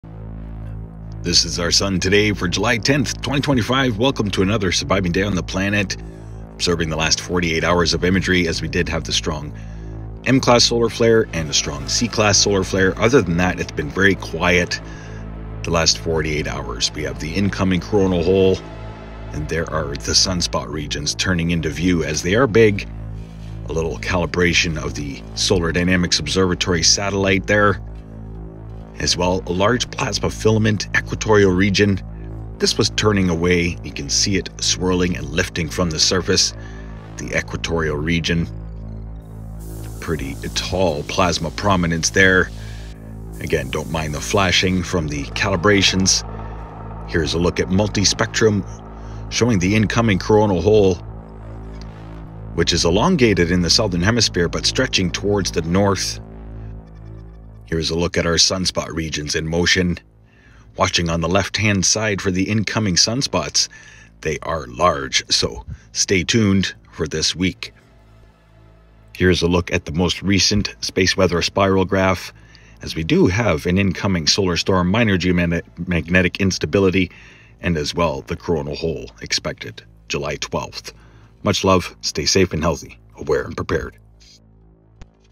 Space Weather Update‼️Incoming Geomagnetic STORM!
July 10 2025
Our sun on July 10th, 2025, showcased a minor M-class solar flare alongside a strong C-class solar flare.
The incoming coronal holes and large sunspot regions are becoming more visible.